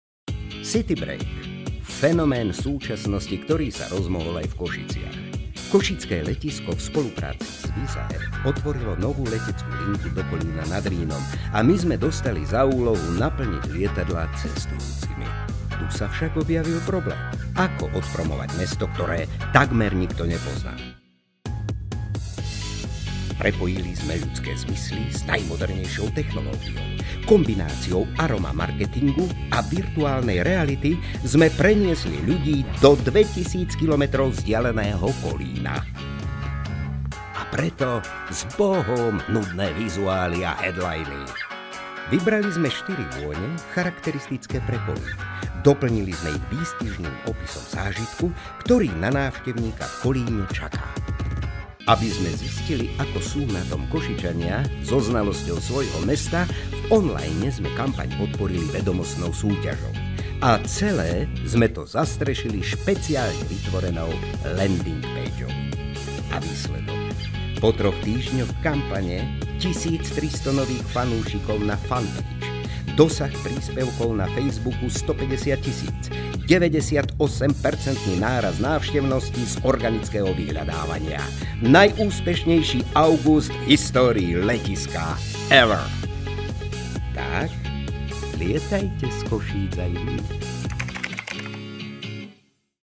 Kampaň Kolín - case study
Naša kampaň pre letisko Košice na propagáciu novej linky z Košic priamo do nemeckého Kolína nad Rýnom dosiahla skvelé výsledky.

Offline prepojený s online-om, voňavé citylighty a virtuálna realita. To všetko sme ponúkli v kampani zameranej na cestovanie z Košíc.